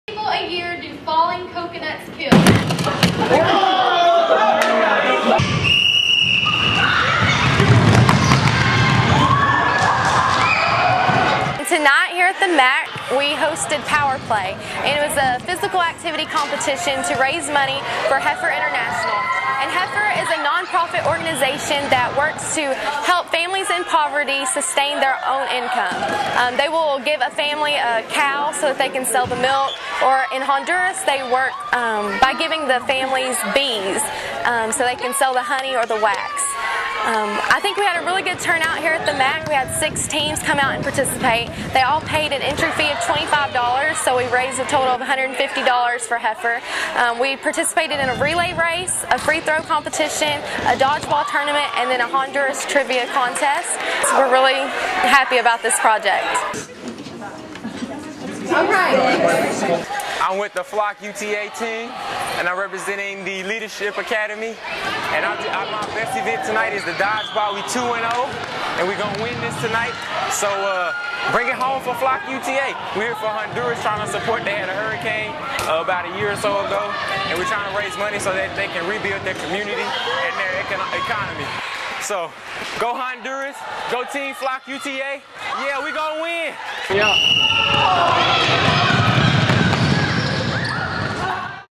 PowerPlay
PowerPlay hosted by Freshmen Leaders on Campus featured relay races, free-throw competition, dodgeball tournament and Honduras trivia, at the University of Texas at Arlington. All proceeds from the event went to Heifer International. April 8, 2009.